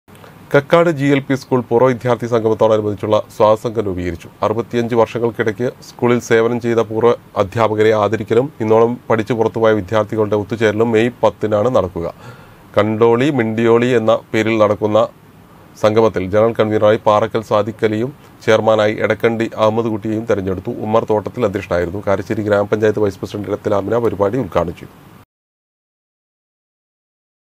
കക്കാട് ജി എൽ പി സ്കൂൾ പൂർവ്വ വിദ്യാർത്ഥി സംഗമത്തോടനുബന്ധിച്ചുള്ള സ്വാഗത സംഘം രൂപീകരിച്ചു
കക്കാട് ജി എൽ പി സ്കൂൾ പൂർവ്വ വിദ്യാർത്ഥി സംഗമത്തോടനുബന്ധിച്ചുള്ള സ്വാഗത സംഘം രൂപീകരിച്ചു. 65 വർഷങ്ങൾക്കിടക്ക് സ്കൂളിൽ സേവനം ചെയ്ത പൂർവ അധ്യാപകരെ ആദരിക്കലും ഇന്നോളം പഠിച്ച് പുറത്തുപോയ വിദ്യാർത്ഥികളുടെ ഒത്തുചേരലും മെയ് 10 നാണ് നടക്കുക